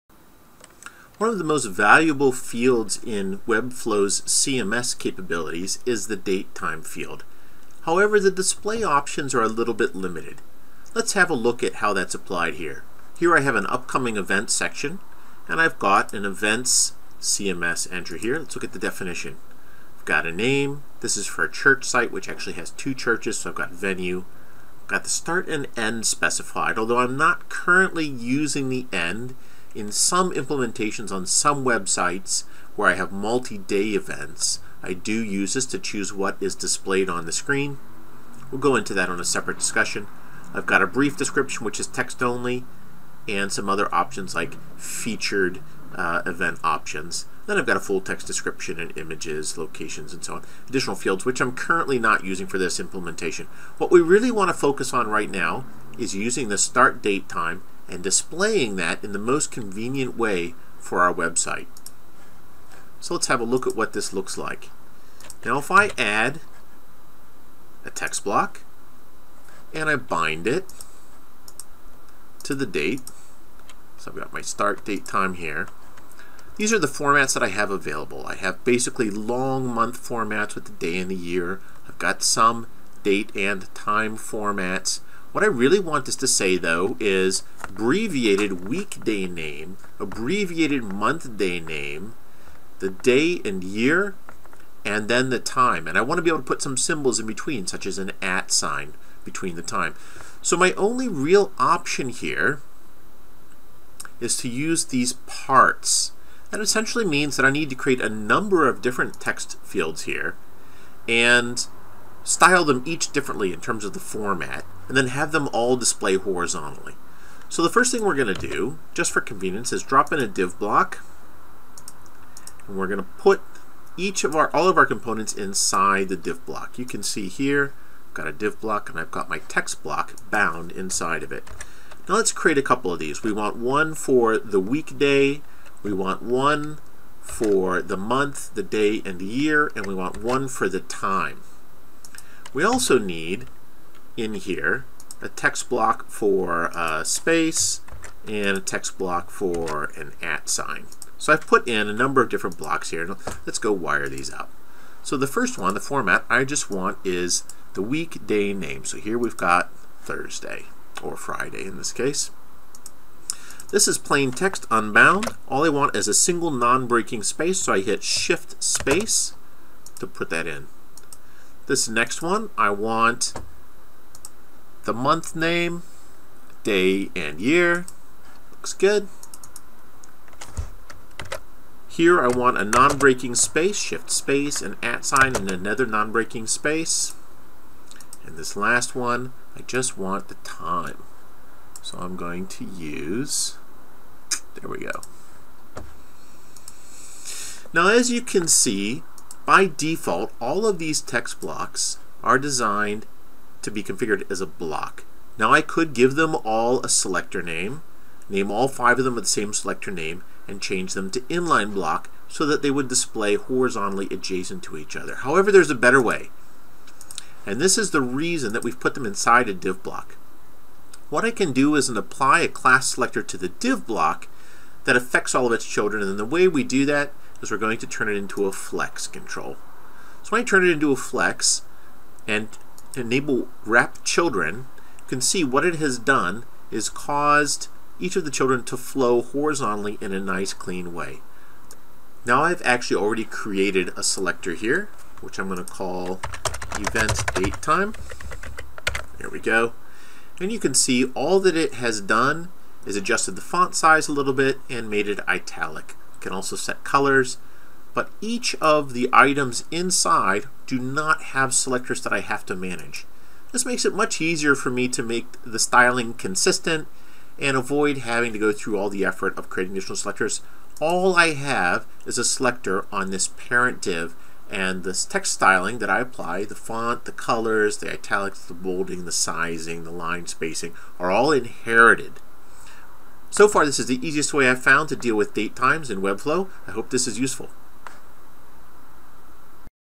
Webflow - How to format CMS dates
Webflow is powerful, and CMS date support is great, but formatting options are limited. Here's the best workaround I've found to conquer those limitations.